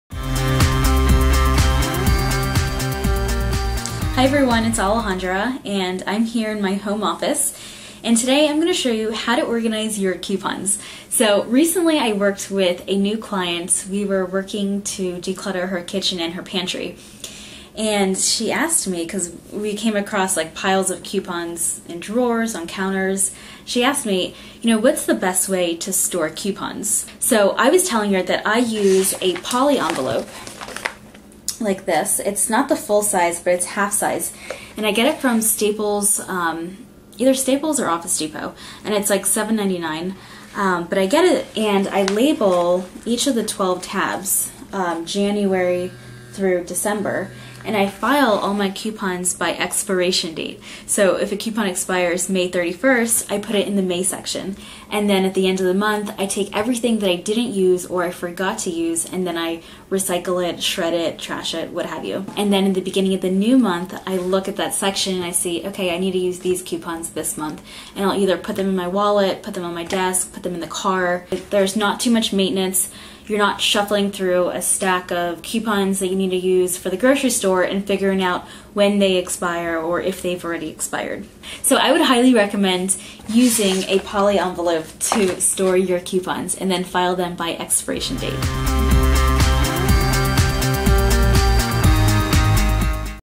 How to Organize Coupons
Sign-up for my FREE Organizing Video Series Here!

Join My Organizing Program Here!

How to organize coupons by expiration date with creative professional organizer Alejandra Costello of